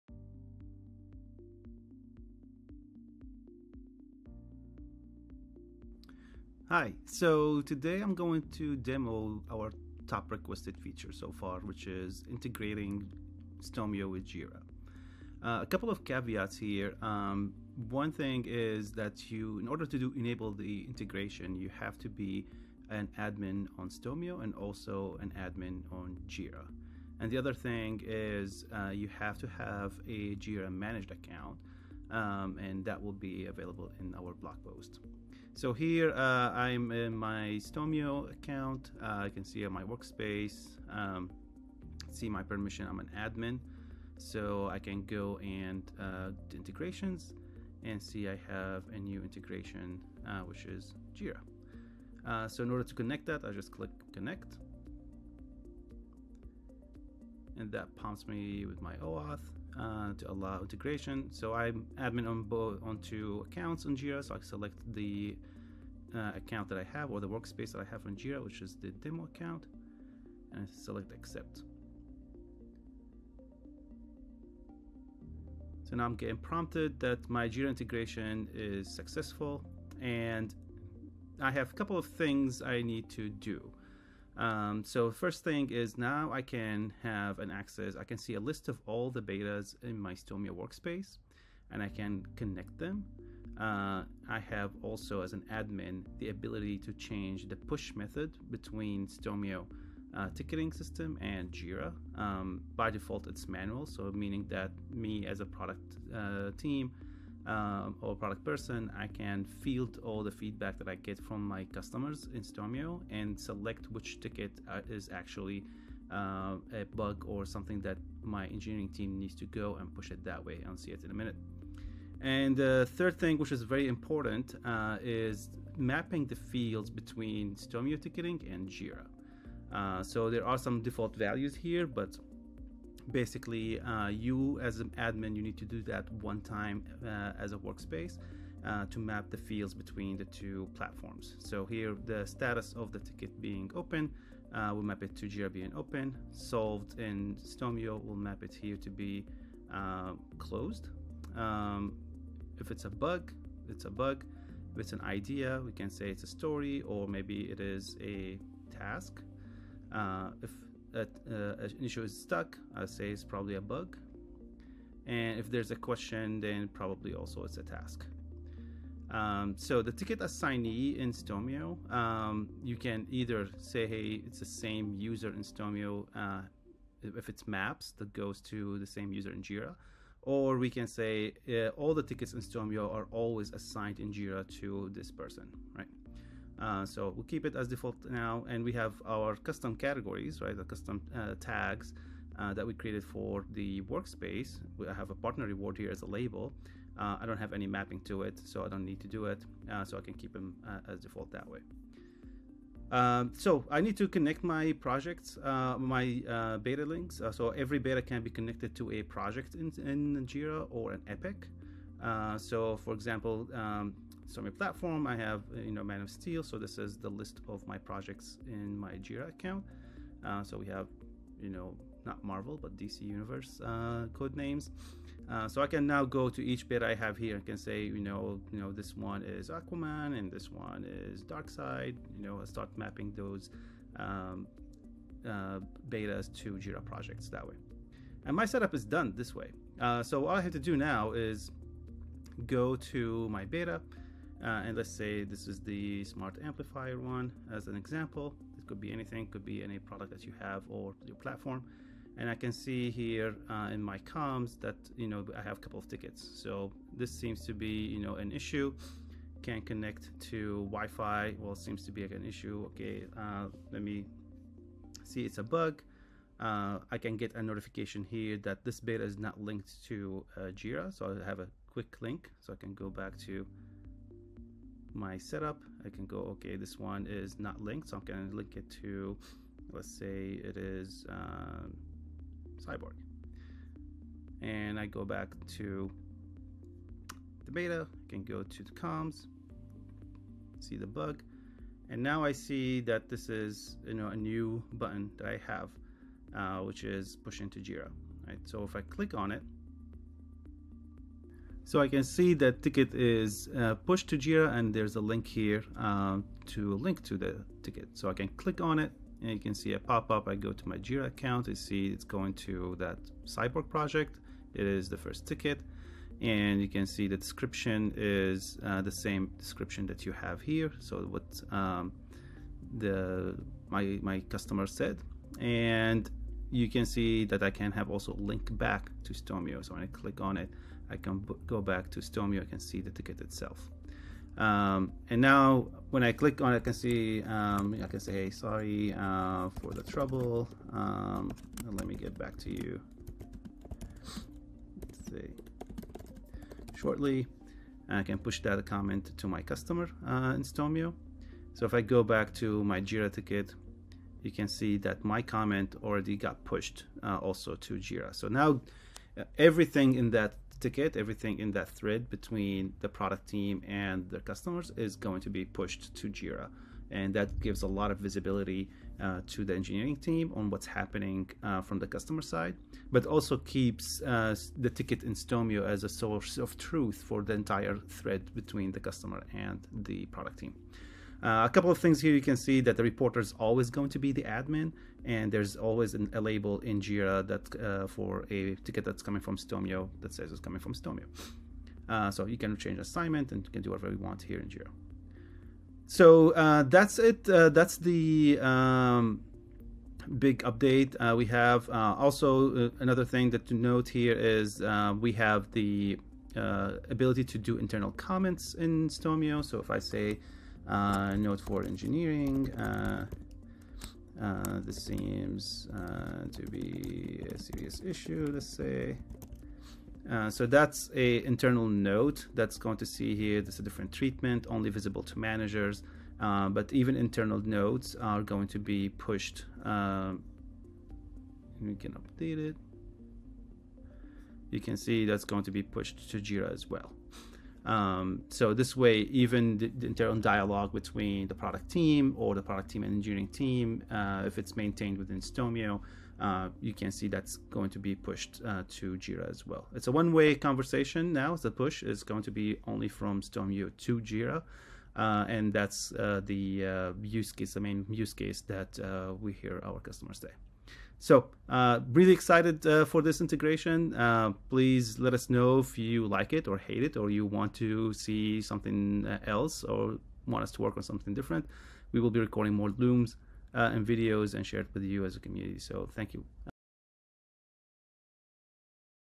How to Integrate Stomio with Jira
Jira is a leading issue tracking platform developed by Atlassian and used by millions of developers worldwide. By integrating Stomio with Jira, you can easily extend your voice of customer to the development team by pushing a ticket in Stomio to an issue in Jira.

Key moments:
0:00 -  Requirements
0:35 - Enable integration
1:12 - Jira set up
3:23 - Beta links
4:46 - Pushing a ticket to Jira
6:42 - Internal comments